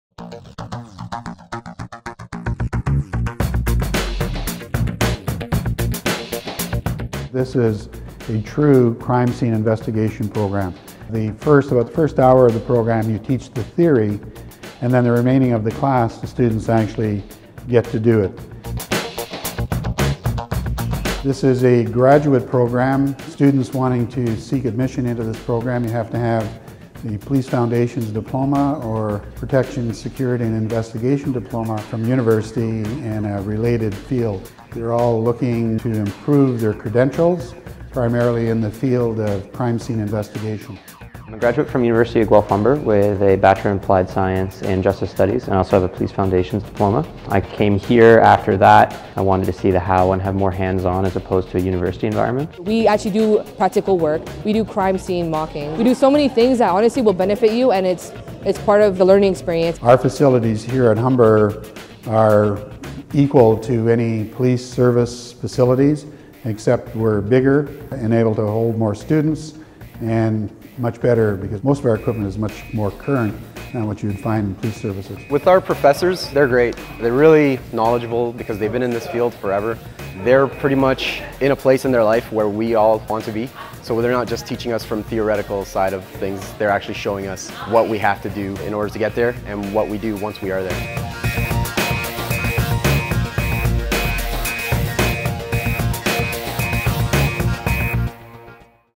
Protection, Security and Investigation: Crime Scene Investigation
Humber's Protection, Security and Investigation: Crime Scene Investigation (CSI) diploma program is an ideal way for university graduates in a related field or Police Foundations  or Law and Security Administration diploma graduates to build on knowledge and skills, enhance credentials and improve their career prospects as CSIs.